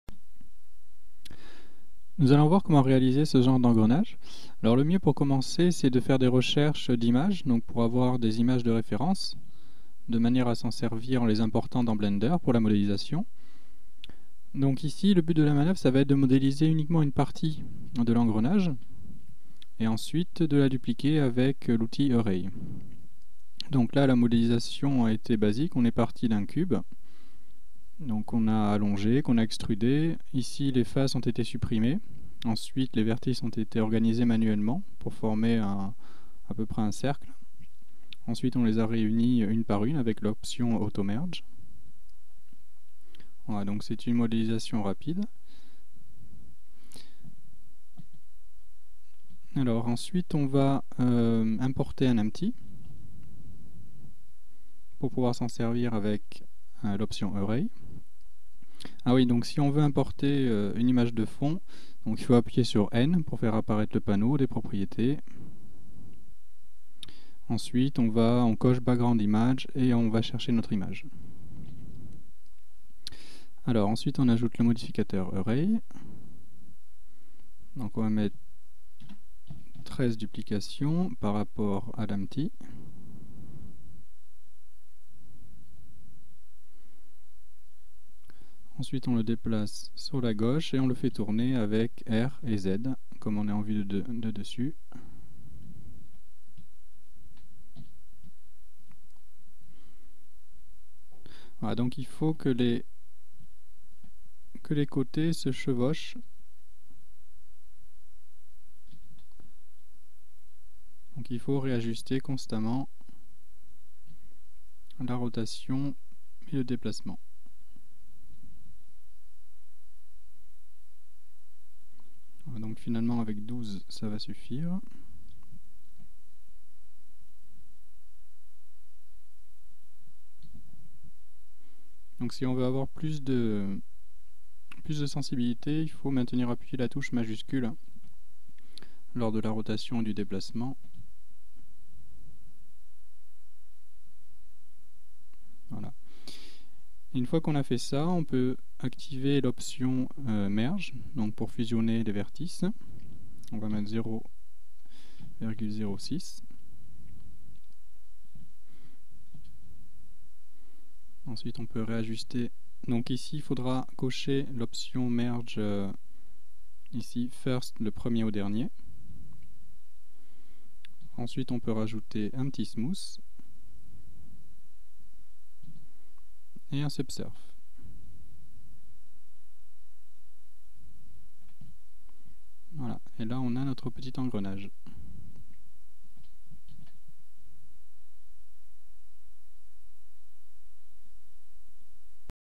Création d'un engrenage (cog)
Création d'un engrenage avec le modificateur array.